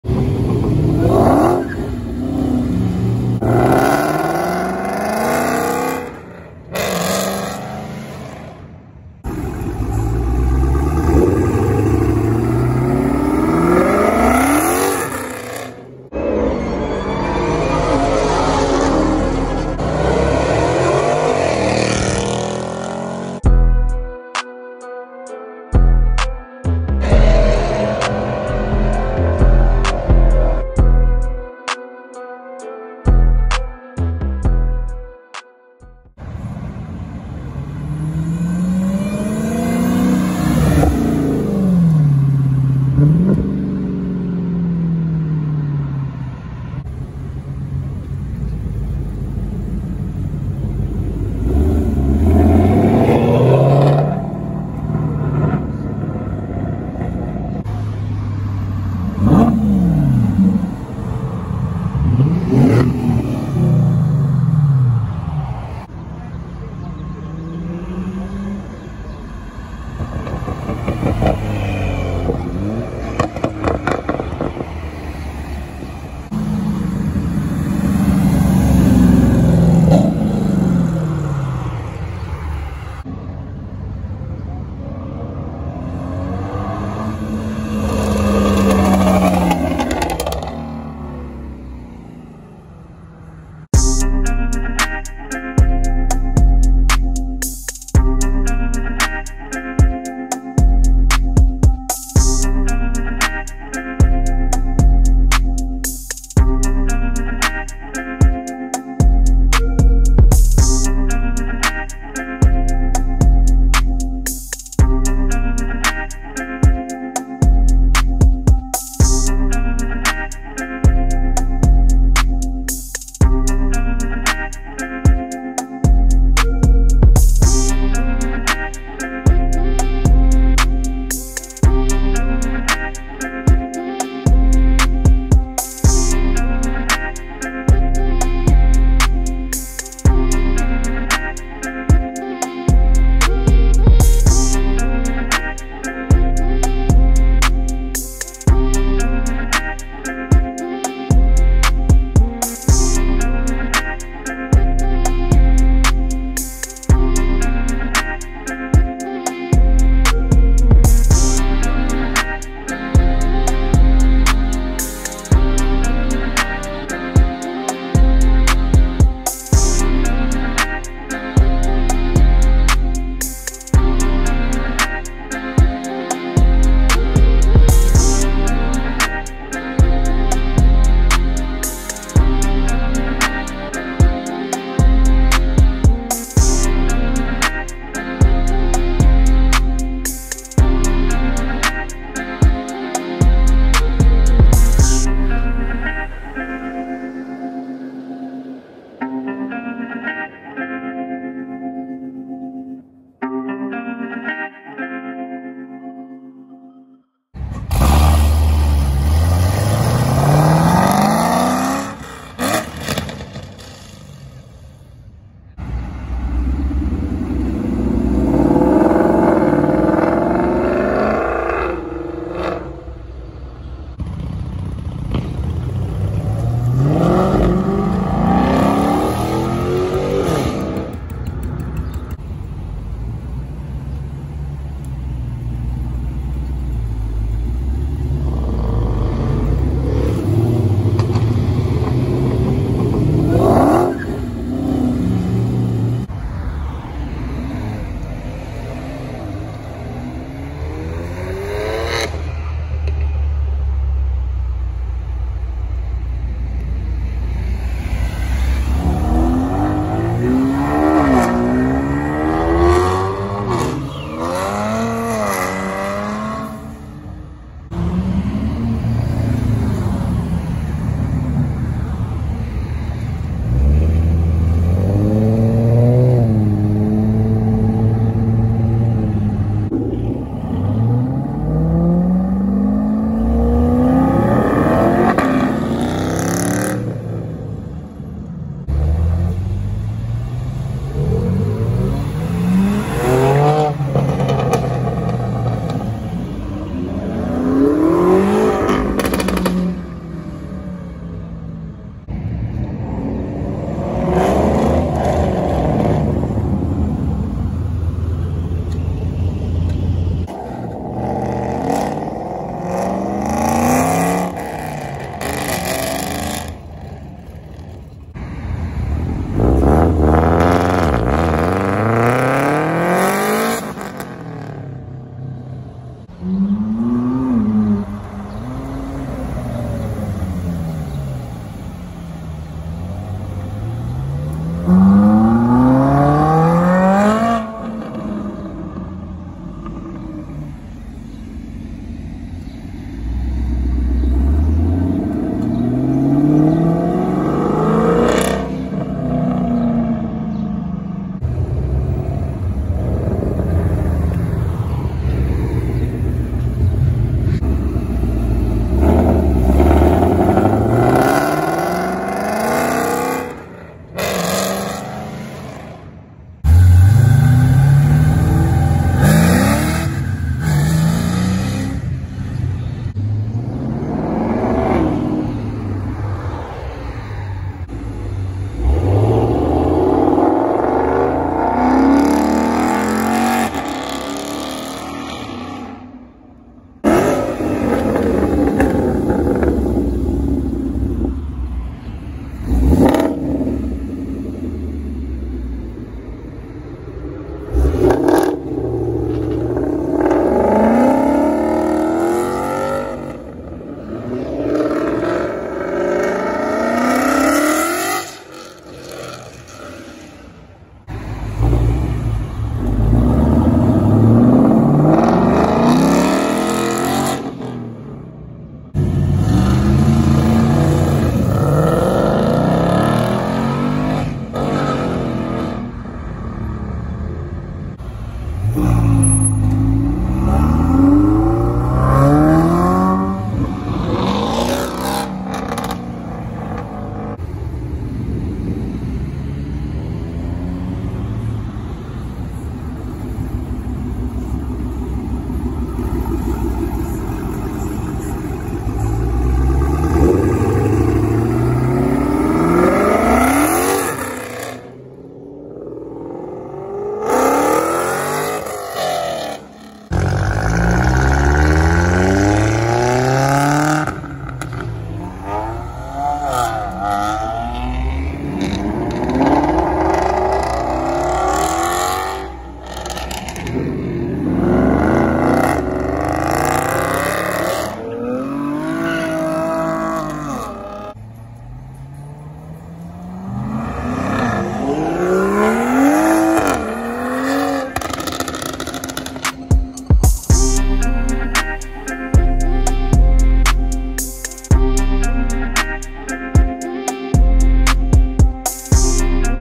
2022 Dodge Challenger Hellcat Redeye Superstock Shows Up At Car Meet
2022 Dodge Challenger Hellcat Redeye Superstock Shows Up At Car Meet

Intro song by: V.I.P.N

Music in video: Epidemic Trap Beats (Jharee)